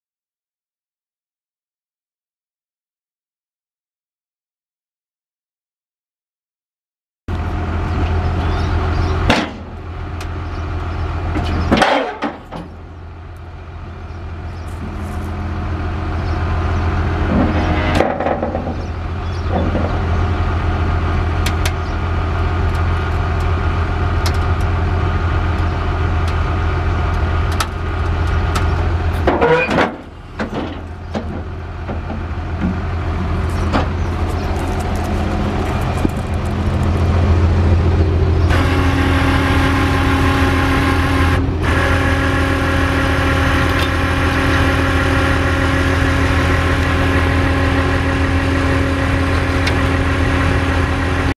1994 Trail-Eze equipment trailer for sale | sold at auction June 11, 2015
1994 Trail-Eze equipment trailer
28&#039;L x 102"W
8&#039;L hydraulic folding dovetail
2&#039;L folding ramps
Wood deck
Hydraulic winch
D-ring tie downs
Spring suspension
Electric brakes
10R17.5 tires
GVWR 39,120 lbs
GAWR 15,000 lbs per axle
1DA42CS18RP011190

It was located in Vinita, OK, and we sold it to the highest bidder for $6,050.